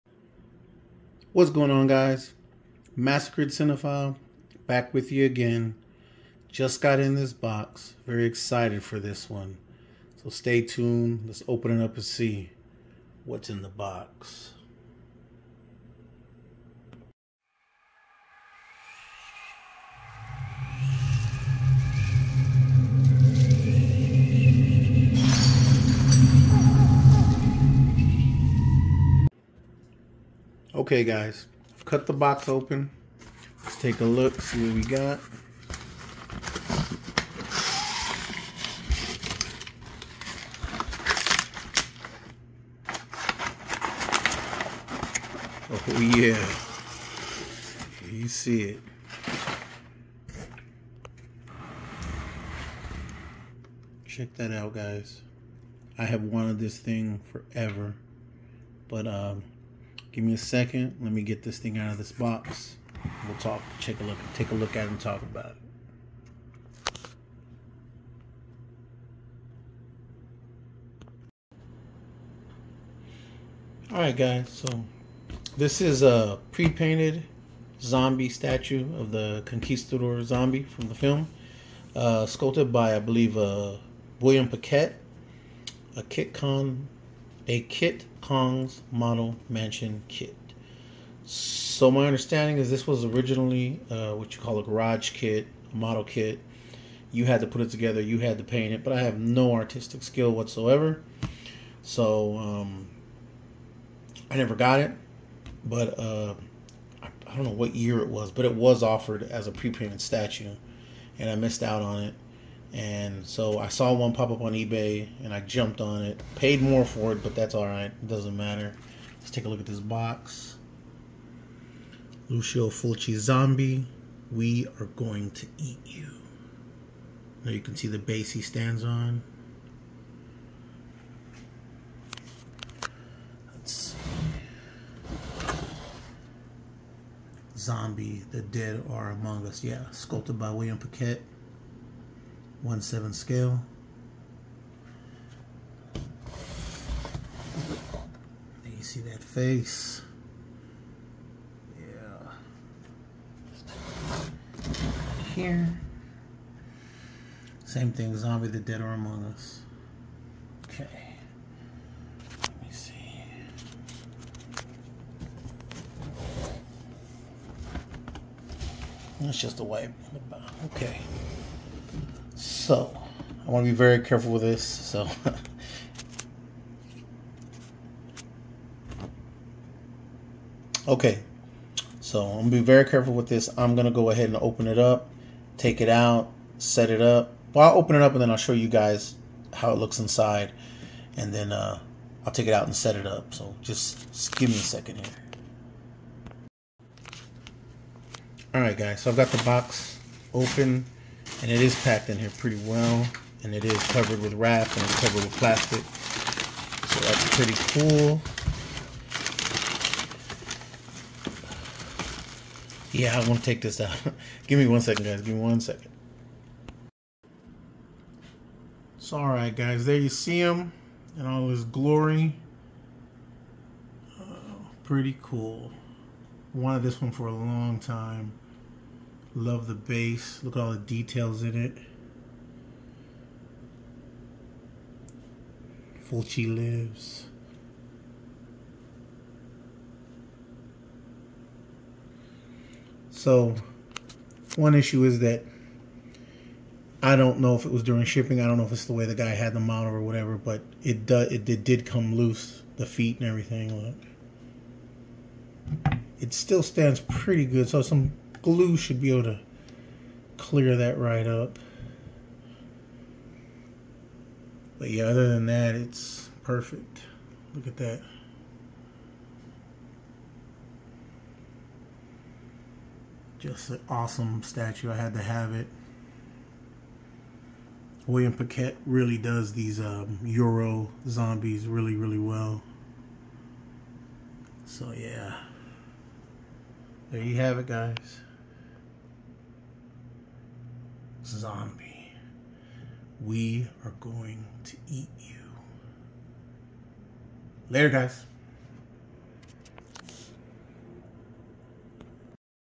KIT KONG'S ZOMBIE PREPAINTED STATUE
Just got in Kit Kong's Zombie prepainted statue. Let's check it out.